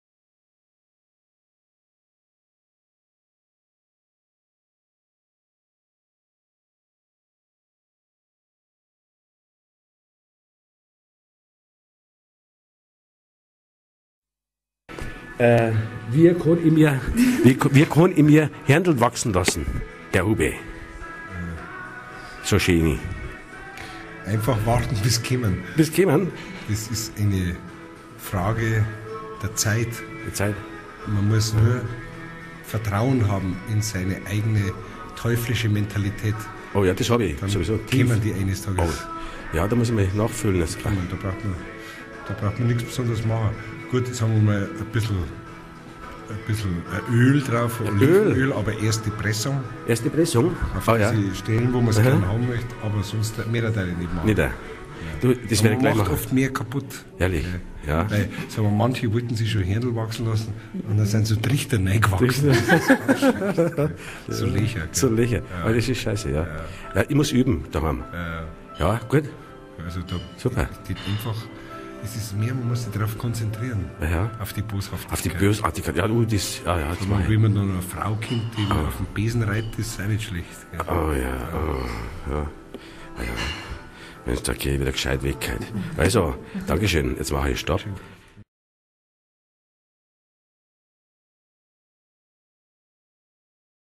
HÖRNER von HUBE
Als der geniale Schauspieler - Kabarettist und Lebenskünstler JÖRG HUBE in Burghausen sein BIO-GRAFFL Programm spielte, war dies ein Juwel. Seinerzeit war ich häuftig unterwegs mit meinem SASS Mikrophon und mit ihm gelang dieses humorvolle Gespräch. Nun, nach so vielen Jahren, habe ich meine Film-Bearbeitung wieder im umfangreichen digitalen Archiv gefunden und deshalb lasse ich das Ganze öffentlich wirken.

In posthumer Verehrung für den grossen Meister, der so ein wunderbares reiches Lebenswerk hinterlassen hat.

Knox G.Stallbauer